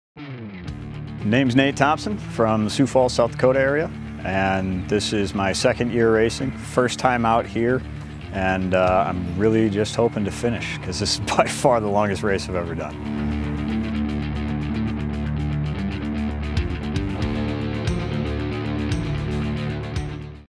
YORR 2020 - Big Sky 200 – Nate Thompson
Interview with Nate Thompson before running the Big Sky 200 in Broadview, MT